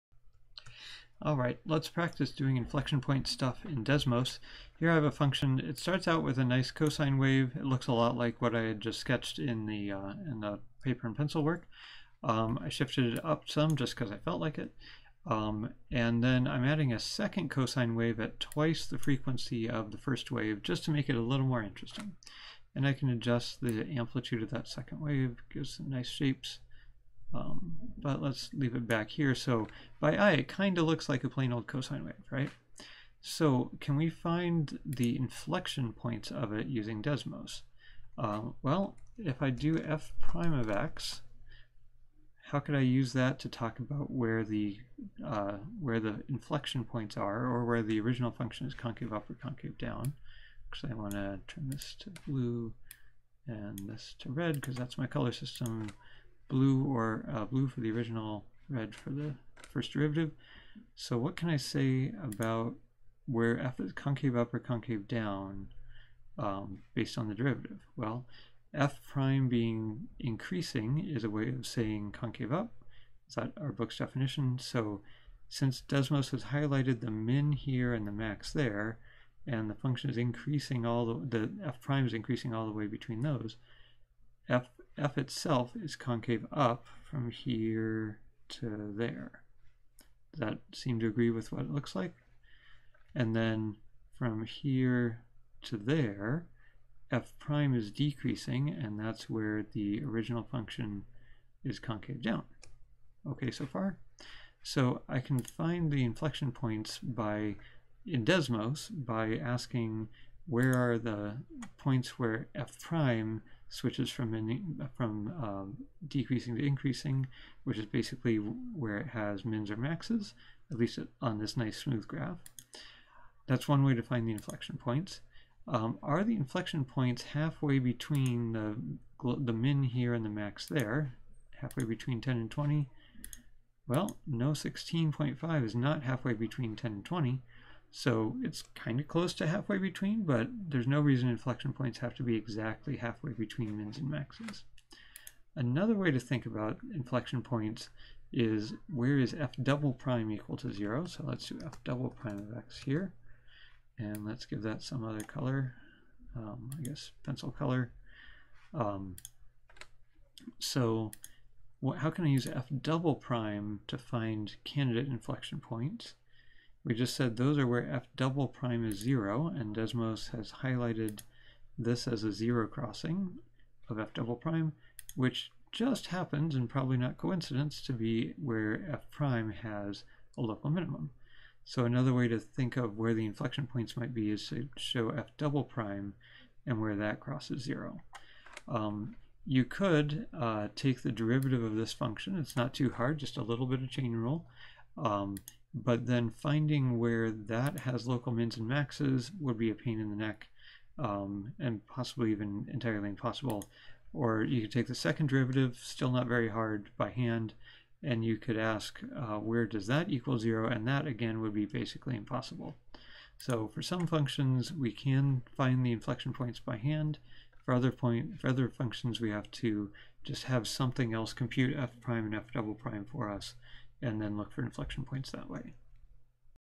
math120 ch34d desmos inflection points
How to find inflection points in Desmos:
a) see where the 1st derivative changes from increasing to decreasing, at a local min or local max of the 1st derivative.
b) see where the 2nd derivative has a value of 0 (crosses the x-axis).
Though this doesn't include finding where the 2nd derivative DNE.